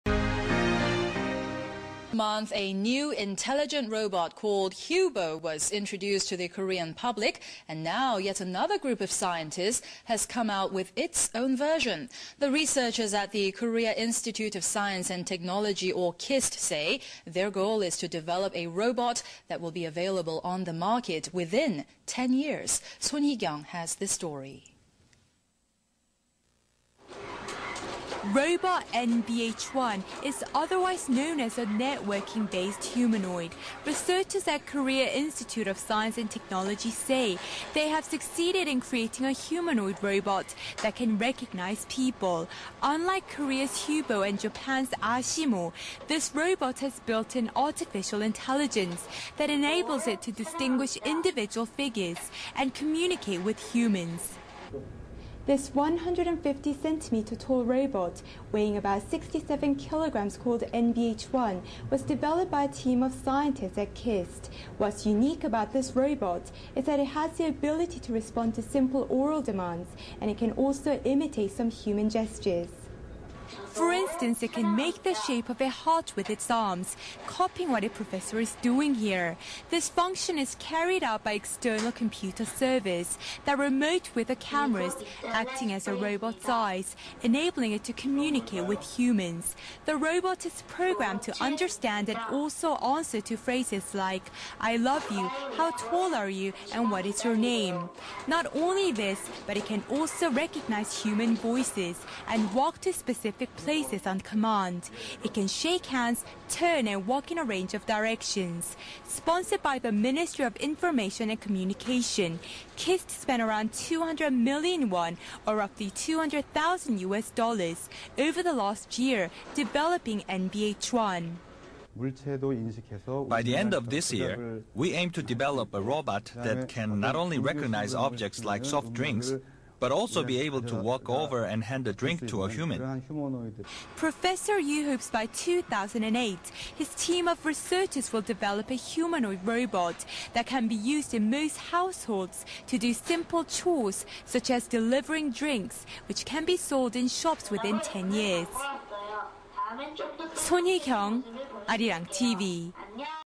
Mahru (2005 news report)
A 2005 news report when KIST's Mahru robot was first unveiled.  If they intend to get a robot to market by 2015 they don't have much time left!